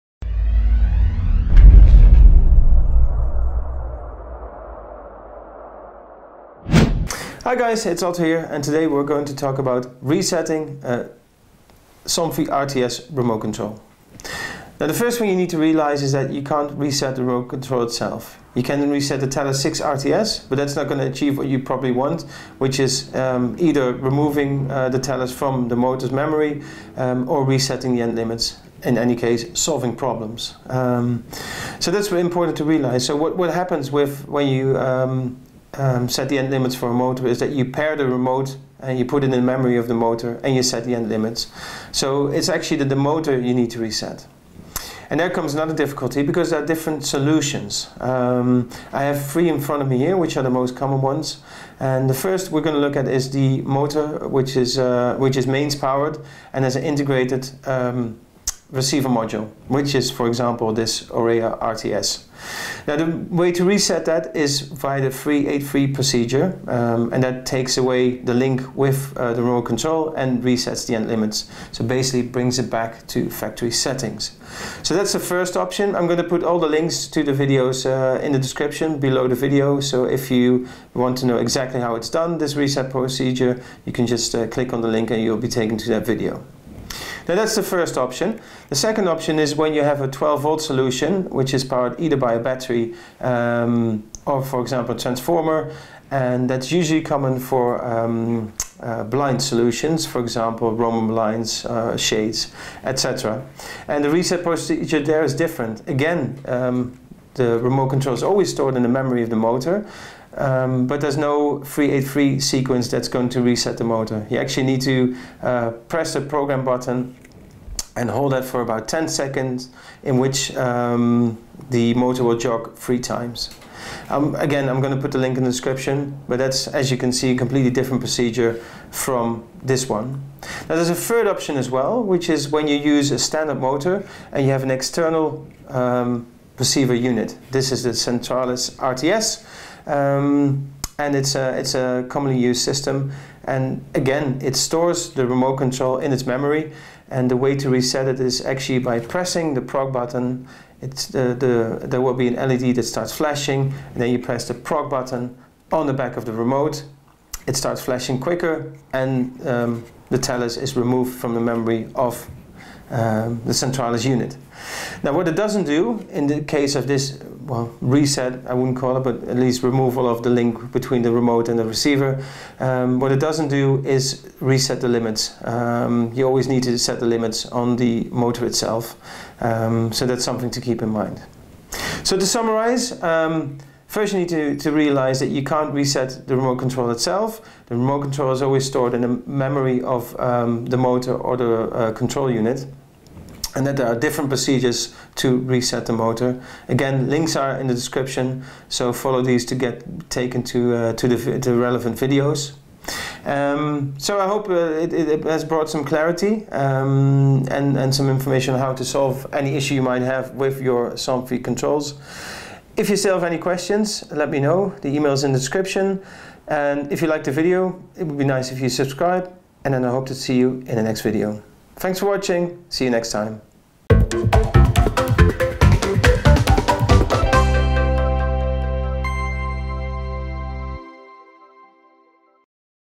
How to reset a Somfy remote control - RTS
When you want to reset your Somfy remote control, here's what you need to know.